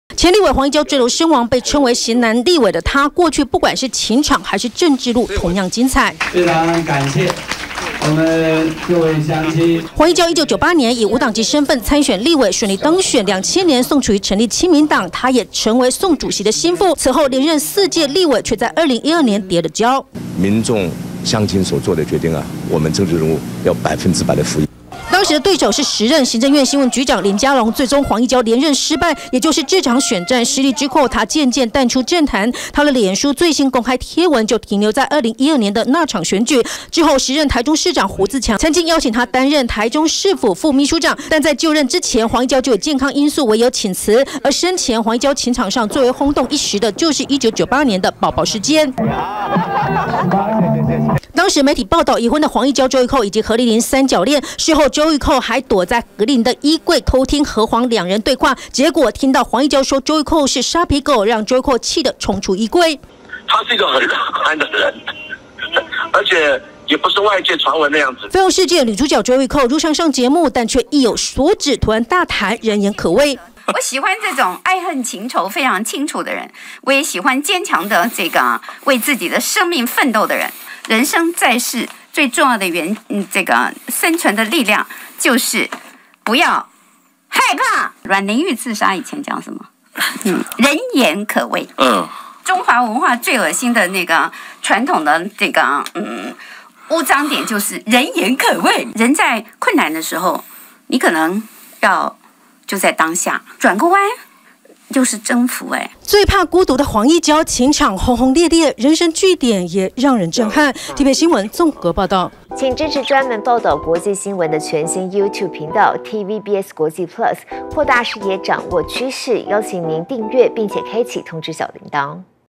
黃義交殞命 周玉蔻突然大談「人言可畏」｜TVBS新聞
黃義交踏入政壇10多年，是親民黨主席宋楚瑜的子弟兵，也曾被譽為型男立委，被視為政治金童的他，情路與政治路同樣轟轟烈烈，1998年的寶寶事件，已婚的他，與周玉蔻以及何麗玲傳出戀情，周玉蔻自爆當時躲在衣櫃，親耳聽到黃義交說她壞話，而今天她如常主持廣播節目，卻突然大聊人生議題，並強調人生在世最重要的就是不要害害怕人言可畏!